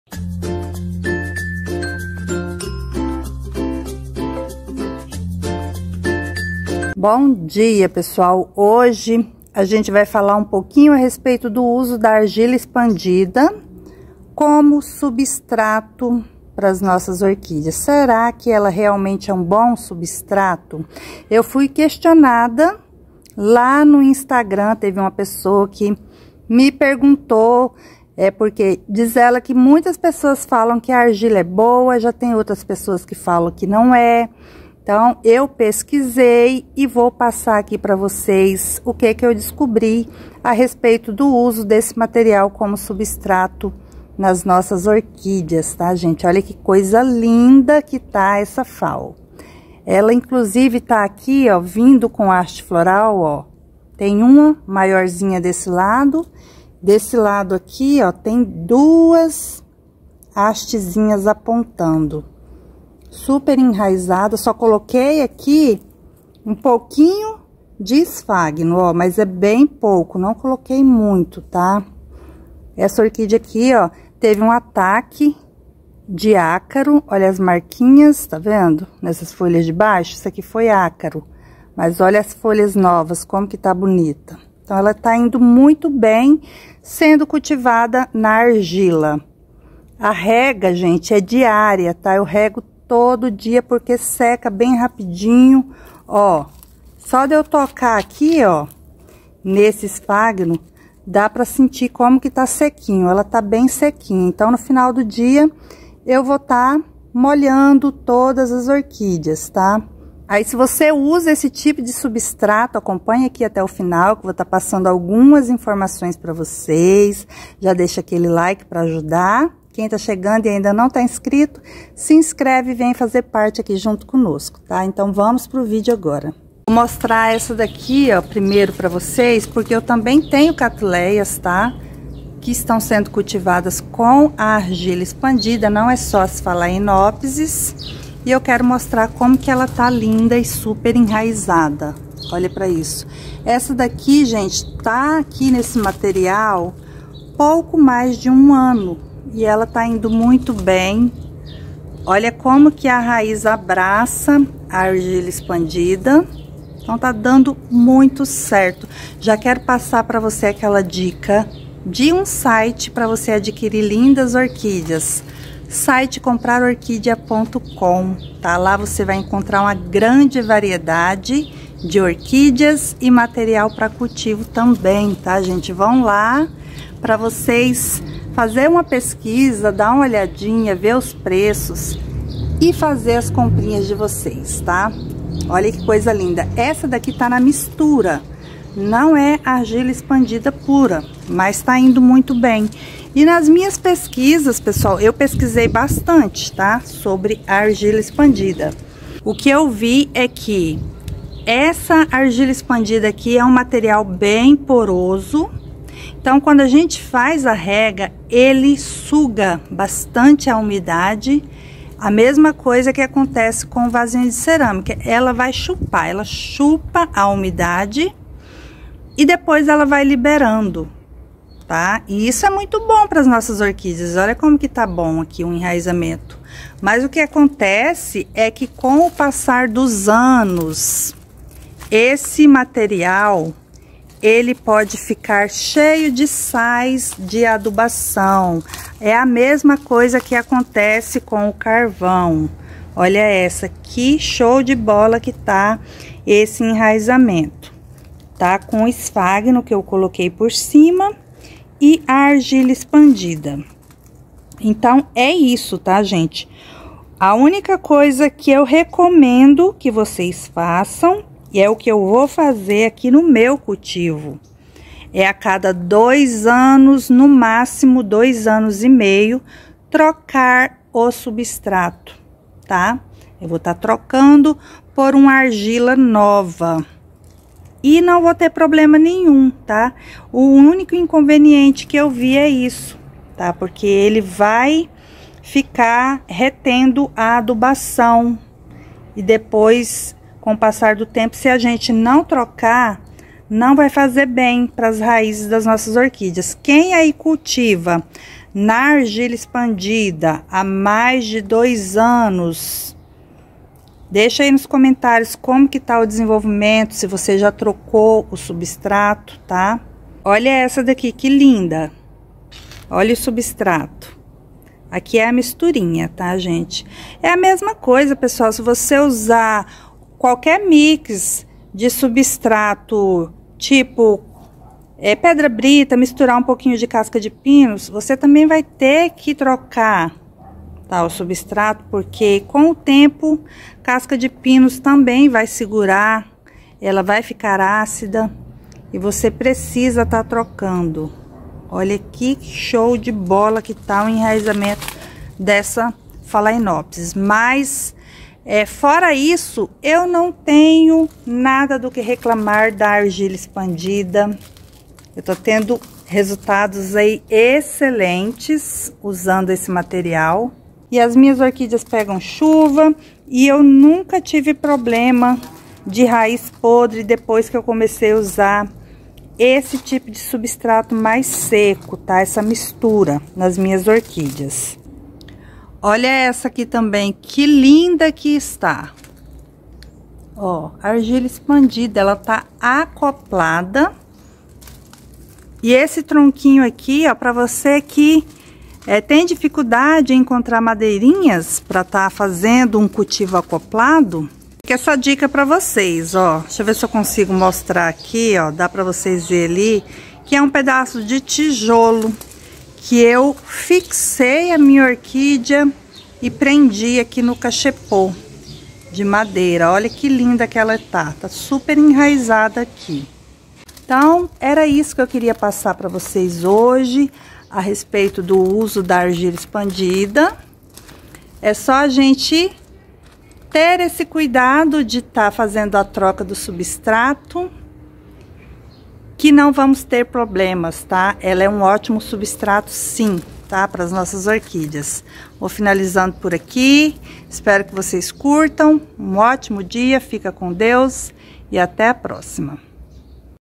ORQUÍDEAS NA ARGILA EXPANDIDA! Veja o resultado e evite problemas futuros
Neste vídeo quero responder uma pergunta sobre o uso da argila expandida como substrato das suas orquídeas.
Veja o resultado do meu cultivo e o que devemos fazer pra não ter problemas.